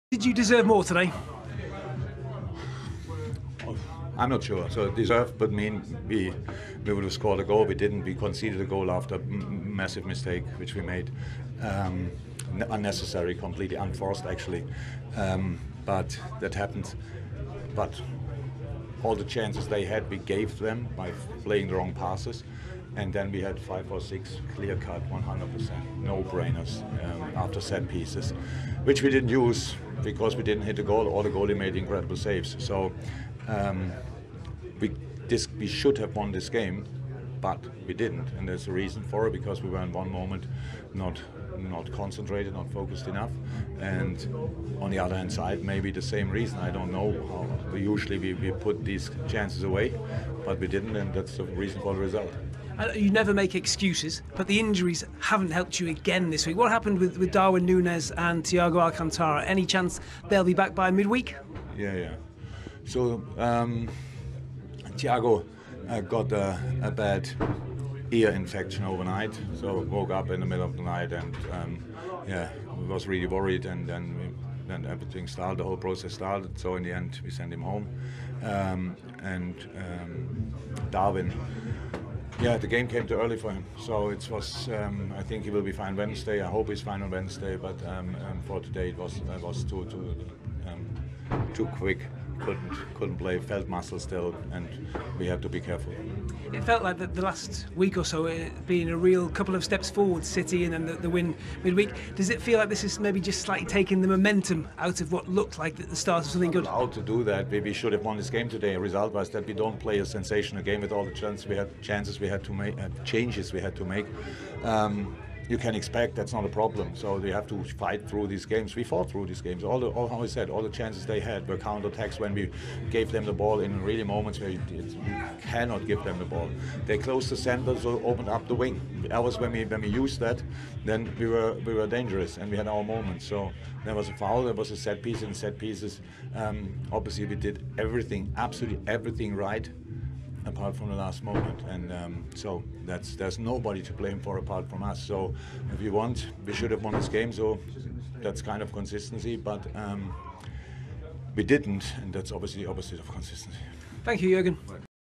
Jürgen Klopp's verdict on Nottingham Forest 1-0 Liverpool
Jürgen Klopp admitted Liverpool only have themselves to blame after they were beaten 1-0 by Nottingham Forest in the Premier League on Saturday.